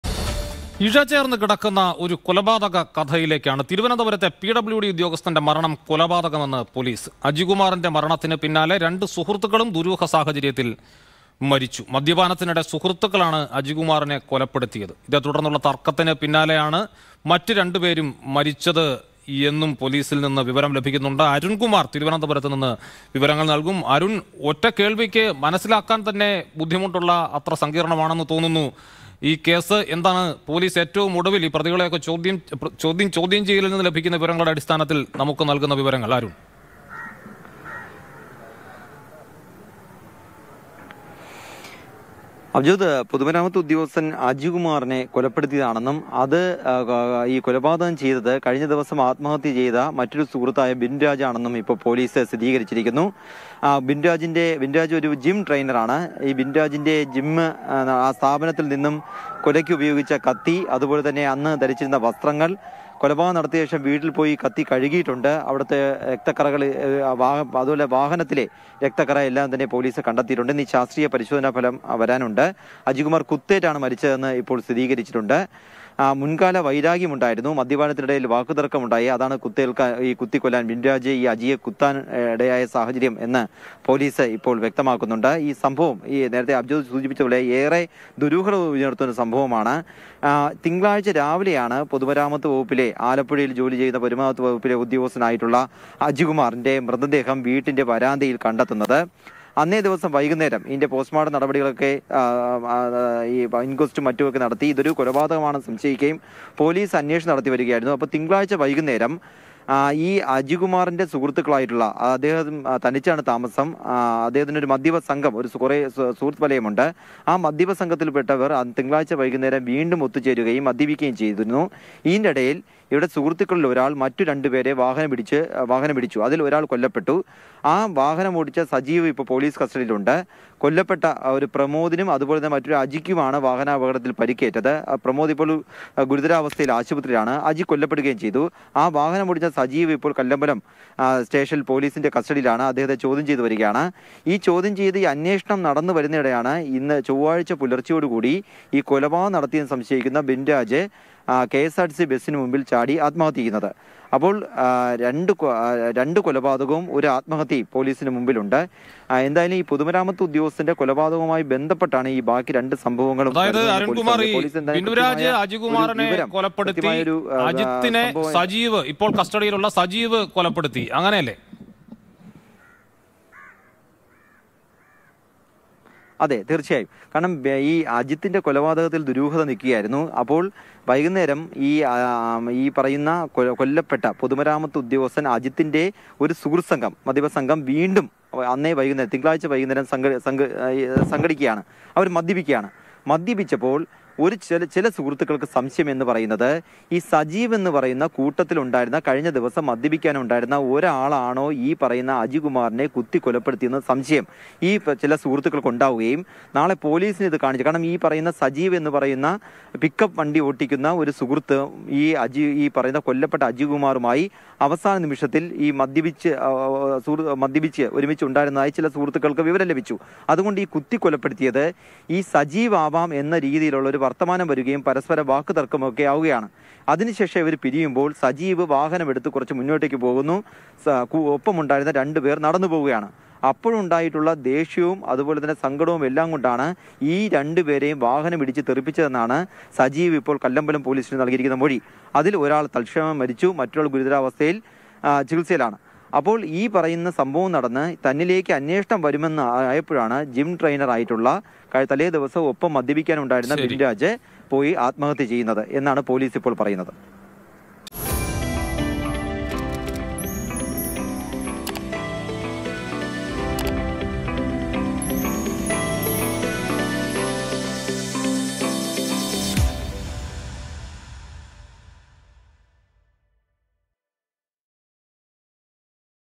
കൊലപാതകം, തര്‍ക്കം, ദുരൂഹ മരണം; കല്ലമ്പലത്തേത് സിനിമയെ വെല്ലുന്ന കൊലപാതക കഥ | Kallambalam Murder
പിഡബ്ല്യുഡി ജീവനക്കാരന്‍ അജികുമാര്‍ കൊല്ലപ്പെട്ടത് കുത്തേറ്റ്. പിന്നാലെ സുഹൃത്തുക്കള്‍ തമ്മില്‍ തര്‍ക്കം. പ്രതികളില്‍ ഒരാള്‍ ബസ്സിടിച്ച് മരിച്ചു, മറ്റൊരാളെ വാഹനമിടിച്ച് കൊലപ്പെടുത്തി. കല്ലമ്പലത്തേത് സിനിമയെ വെല്ലുന്ന കൊലപാതക കഥ